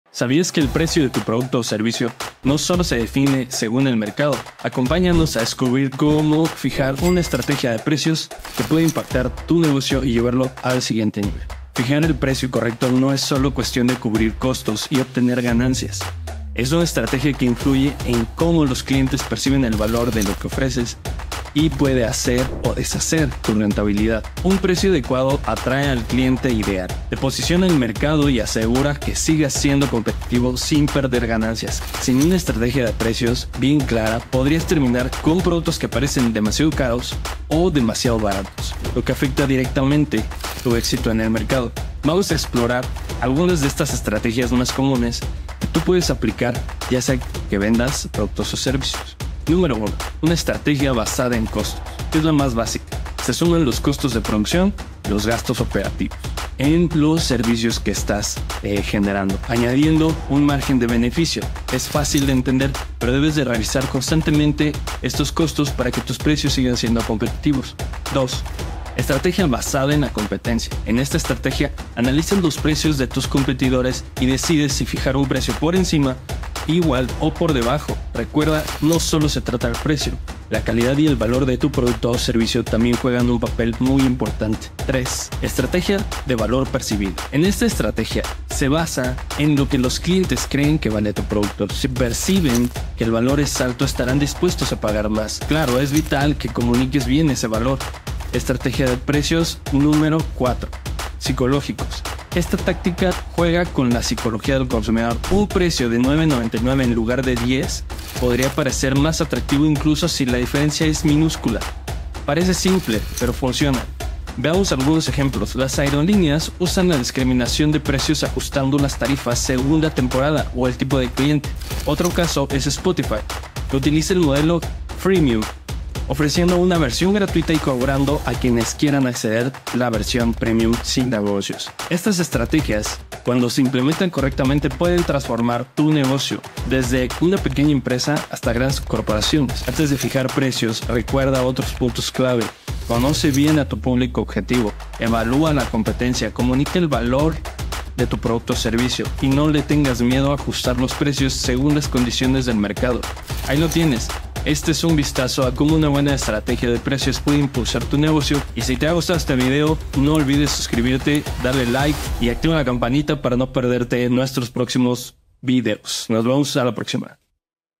¡Cómo fijar el precio perfecto para tu producto o servicio! | Estrategias de precios clave
¿Sabías que el precio de tu producto o servicio puede definir el éxito de tu negocio? En este video te mostramos cómo una estrategia de precios bien planificada puede marcar la diferencia entre ganar y perder en el mercado.

🔍 ¿Por qué es tan importante una estrategia de precios?
Aprenderás que fijar precios no es solo cuestión de cubrir costos y obtener ganancias, ¡es toda una estrategia! Descubre cómo los precios influyen en la percepción de valor de tus clientes y cómo una buena estrategia puede posicionarte mejor en el mercado.

💡 Estrategias clave para fijar precios:

Estrategia basada en costos 💵
Estrategia basada en la competencia ⚖️
Estrategia de valor percibido 🌟
Estrategia de precios psicológicos 🧠
Además, te mostramos ejemplos de empresas como aerolíneas y Spotify, que usan estas tácticas para maximizar su éxito.

📊 Consejos prácticos para tu negocio:

Conoce a tu público objetivo
Evalúa tu competencia
Comunica bien el valor de tu producto o servicio
Ajusta los precios según el mercado
¡No te lo pierdas! Si quieres aprender a fijar precios que impulsen tu negocio, este video es para ti.